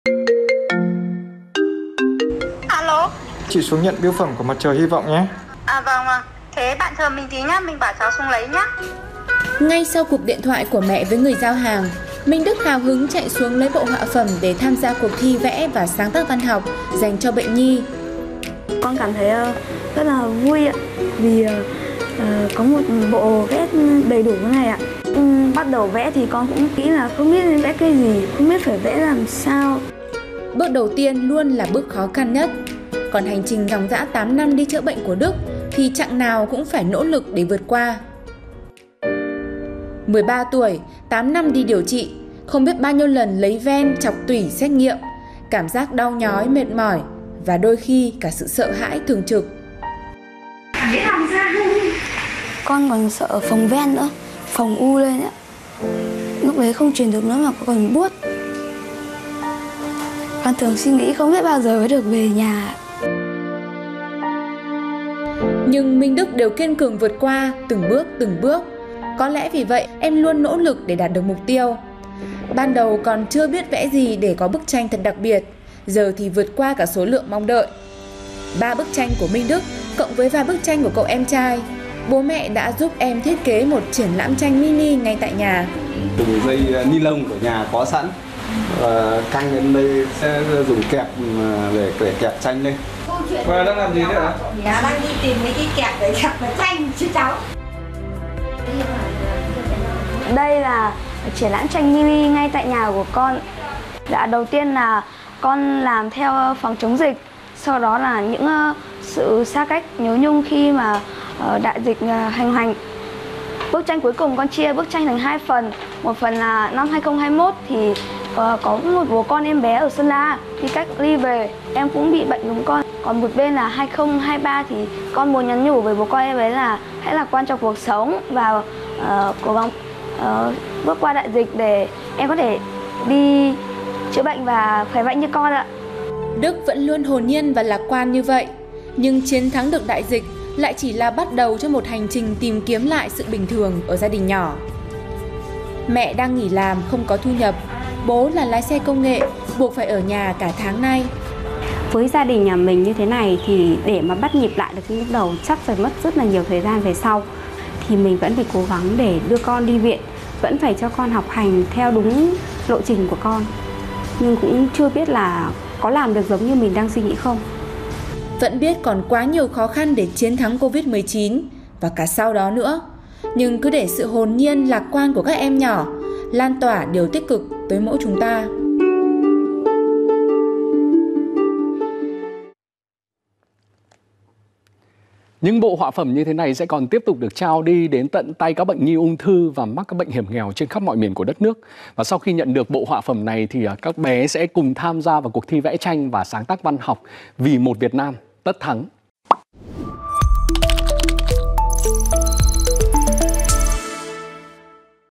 Triển lãm tranh nhỏ của cậu bé 8 năm chống chọi với ung thư | VTV24
Hãy cứ để sự lạc quan của các em nhỏ lan tỏa những điều tích cực tới mỗi chúng ta.